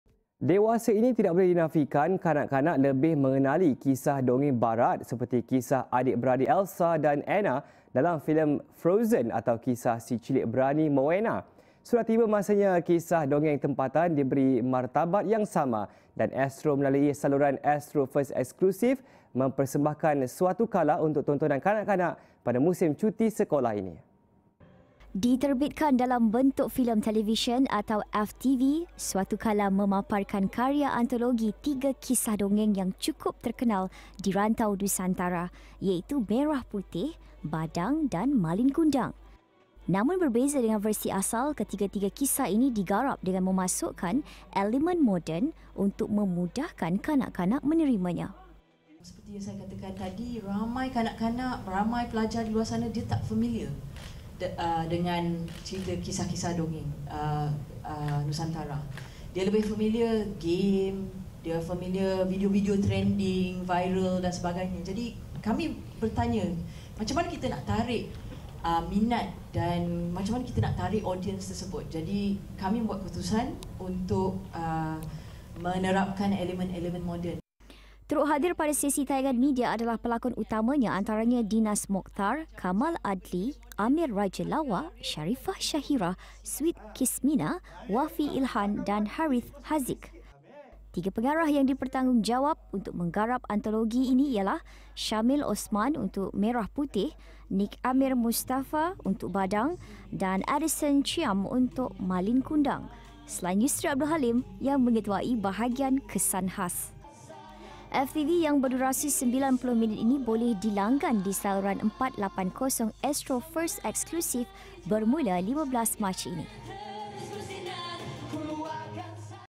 'Suatukala' angkat martabat dongeng Nusantara
Dewasa ini, tidak boleh dinafikan kanak-kanak lebih mengenali kisah dongeng barat, seperti kisah adik beradik Elsa dan Anna dalam filem Frozen, atau kisah si cilik berani Moana.